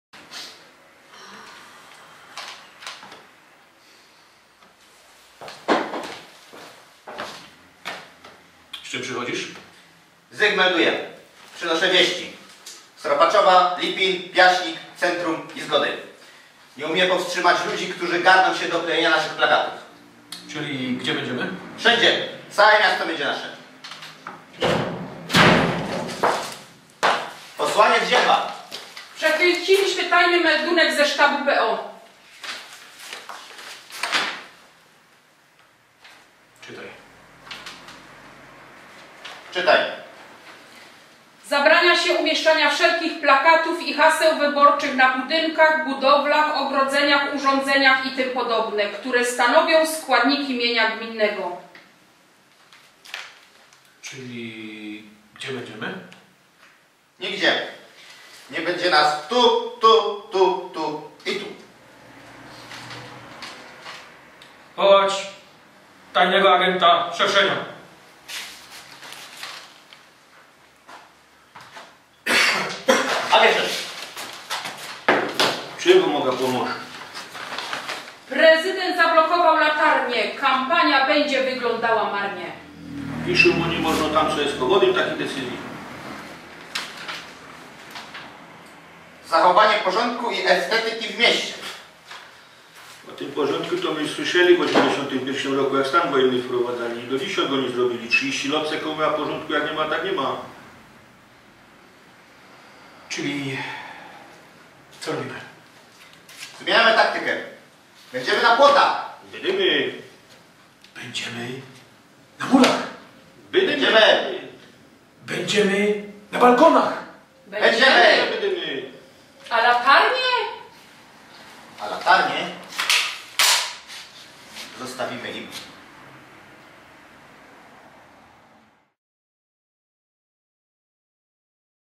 Prezydent zablokował latarnie - sztab wyborczy PiS
Narada w sztabie PiS Świętochłowice po zakazie prezydenta o umieszczaniu reklam wyborczych na infrastrukturze miejskiej.